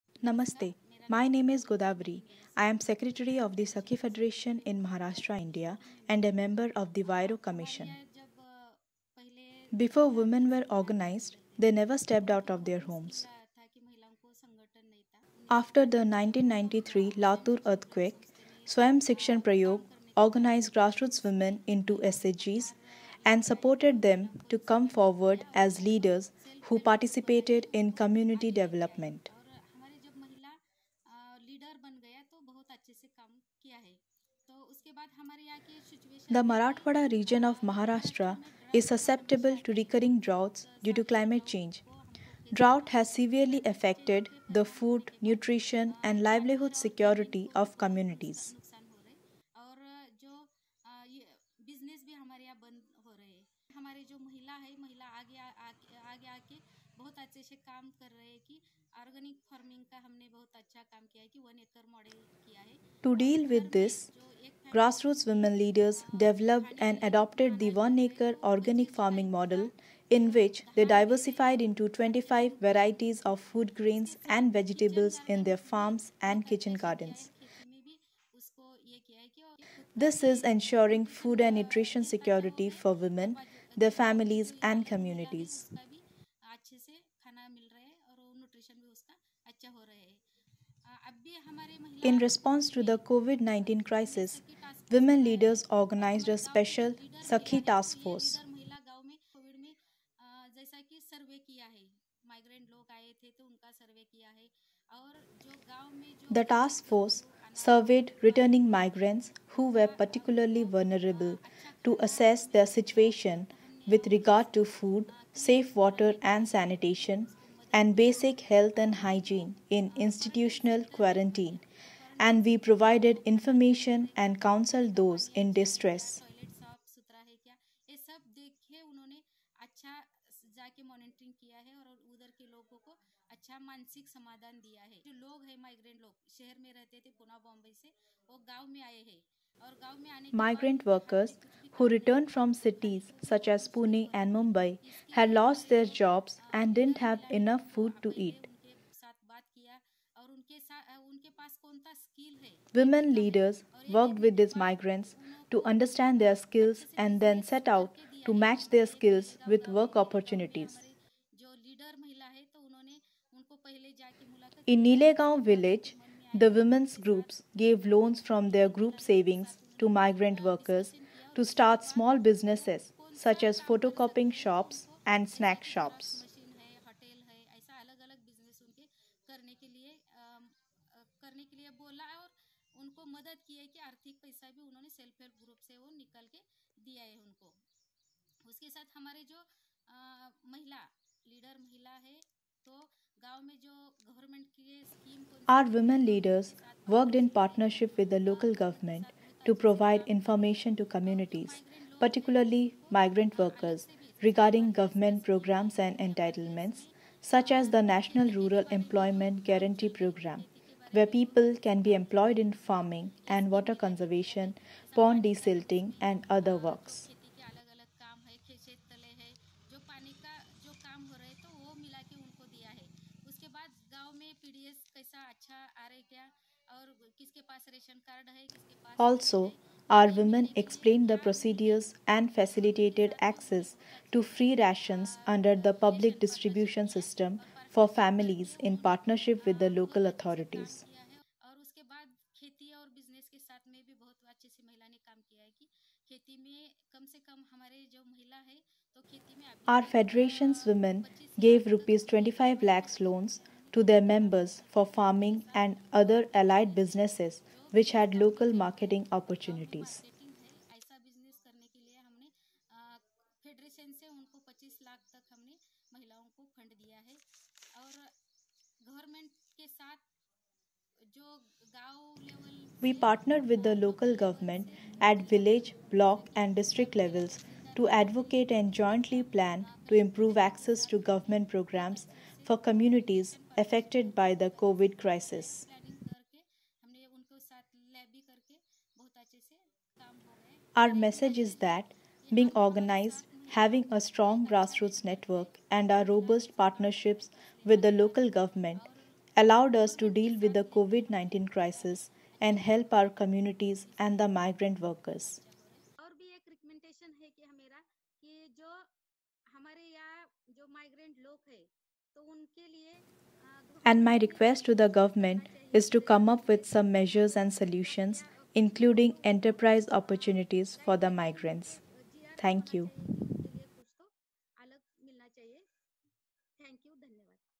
Dealing with COVID-19, from the community up
Godavari Dange, secretary of the Sakhi Federation in Maharashtra, India, and a member of the Huairou Commission, shares the story of her women's association.

Dange shared the story during a webinar held in advance of the 14th International Conference on Community-based Adaptation (CBA14), which puts practitioners’ solutions to climate change first.

With the Marathwada region of Maharashtra susceptible to recurring droughts due to climate change, Dange highlights how grassroots women leaders developed and adopted organic farming models to ensure food and nutrition security for women, their families and communities.

She also outlines how partnerships have been forged with the local government at village, block and district levels to advocate and jointly plan to improve access to government programmes for communities affected by the COVID-19 crisis.

The webinar was organised by Red Cross Red Crescent Climate Centre, Practical Action, Huairou Commission, Green Africa Youth, IUCN NL, Climate Justice Resilience Fund, CARE, Global Resilience Partnership, Environmental Management for Livelihood Improvement Bwaise Facility and the International Institute for Environment and Development.